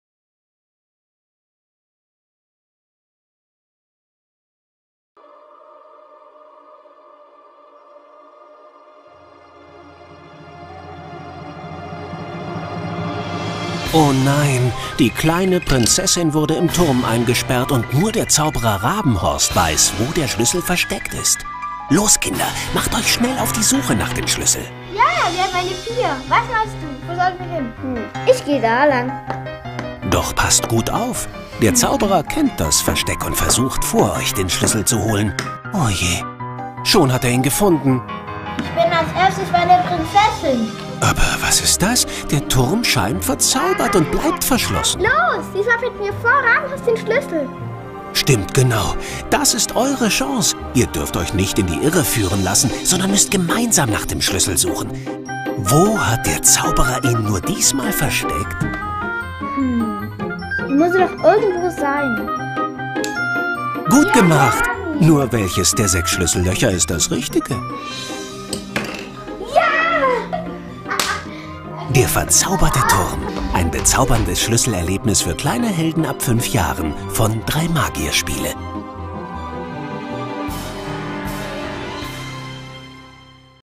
Настольная игра «Заколдованная башня»
Настольная игра «Заколдованная башня» Drei Magier – захватывающее приключение, в котором сталкиваются принцесса, злой волшебник и обычный мальчик Робин. Как привести эту сказку к счастливому концу?